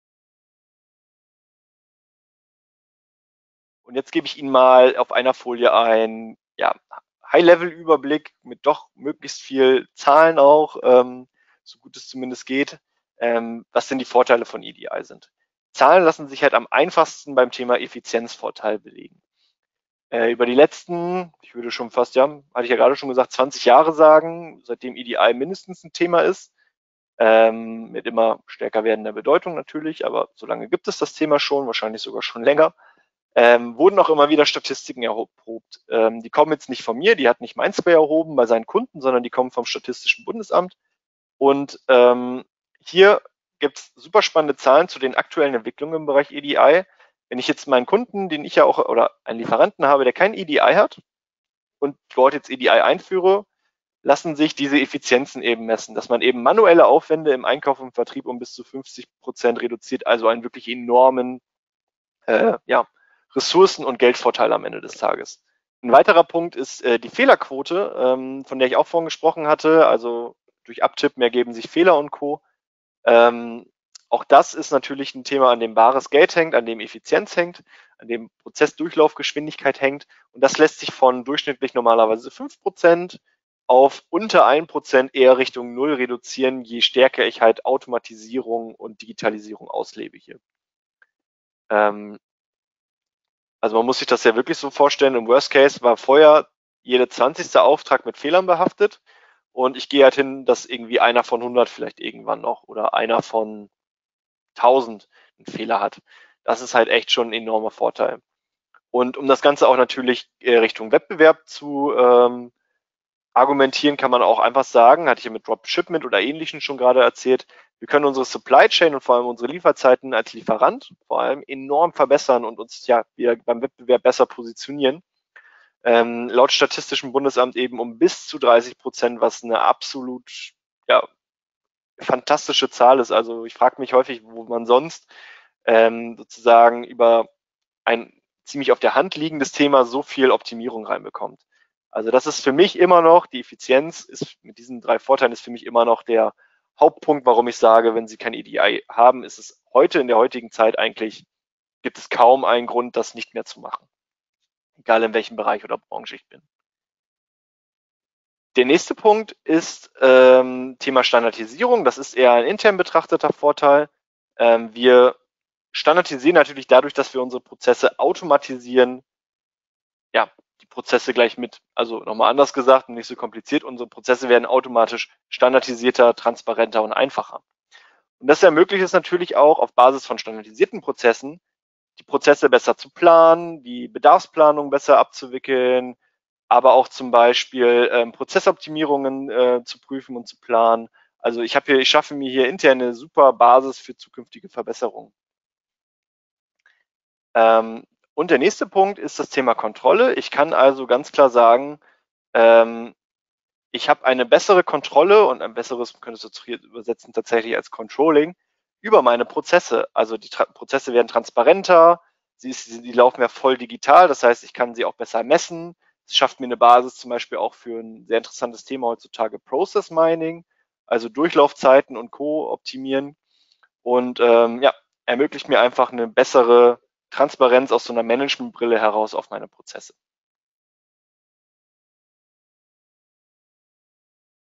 Vorteile von EDI
Was sind die zentralen Vorteile von EDI und EDI-Anbindungen? Unser SAP-Integrations-Experte beantwortet diese Fragen in diesem Video.

Sie haben Fragen?